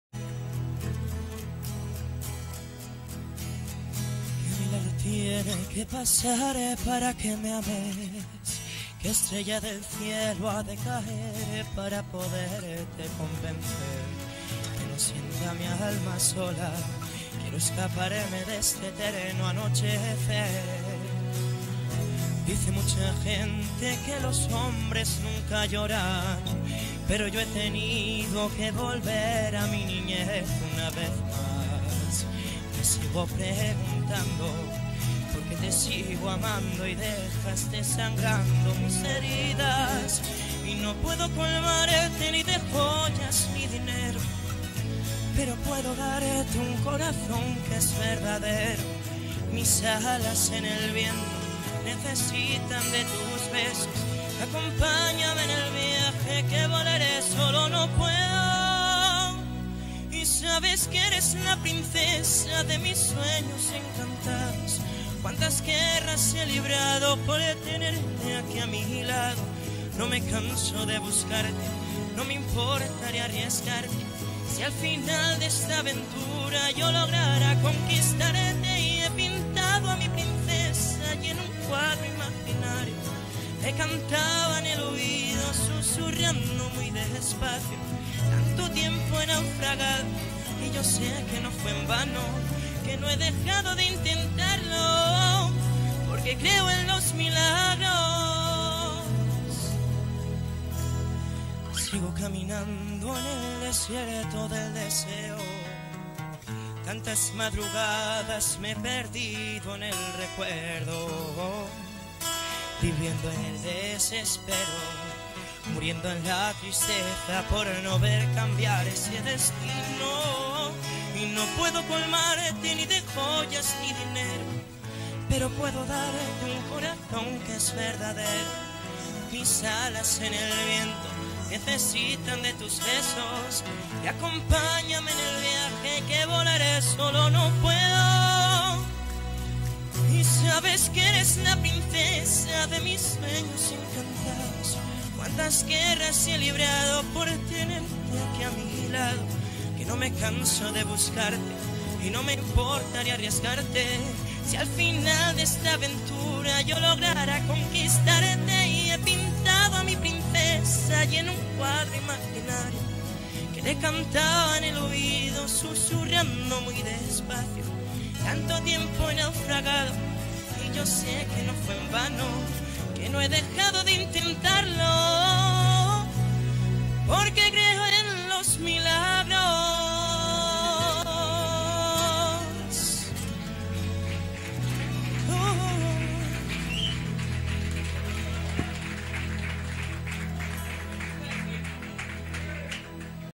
VII CAMPEONATO DE LA RIOJA DE KARAOKE EN LOGROÑO.
Enrique Ruiz en la séptima sesión clasificatoria de la séptima edición del Campeonato de La Rioja de Karaoke.